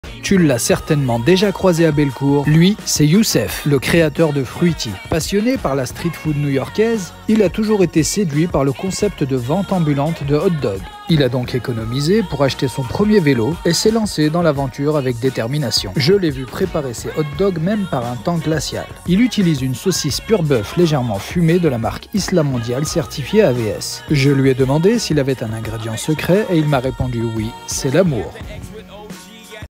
Youcef : Le Vendeur de Hot-dog Ambulant à Lyon
Suivez @fruity_lyon sur Instagram et @fruitylyon sur TikTok pour connaître ses déplacements !

Youcef est un vendeur ambulant de hot-dogs, halal certifié AVS, vous pouvez le retrouver les mardis et jeudis à Charpennes, les mercredis et samedi à bellecour (rue de la république) et le dimanche aux abords du parc de la tête de d’or.